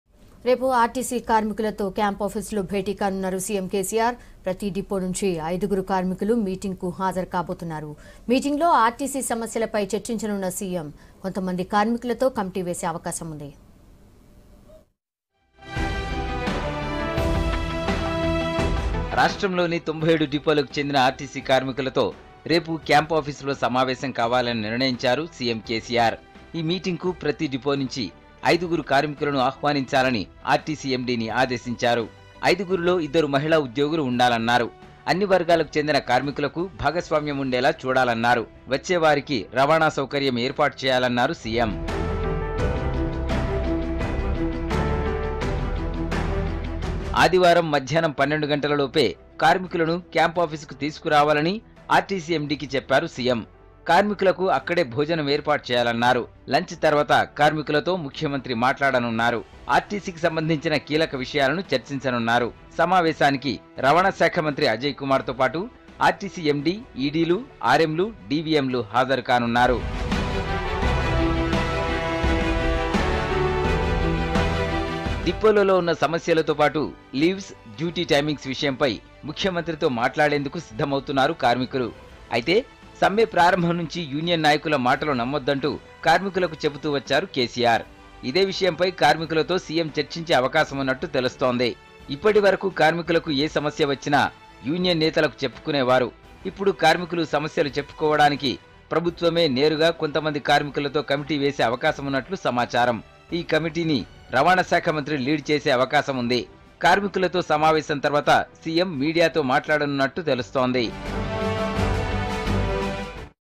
CM KCR To Meet TSRTC Employees Tomorrow | TSRTC Issue | V6 Telugu News
CM KCR To Meet TSRTC Employees Tomorrow | TSRTC Issue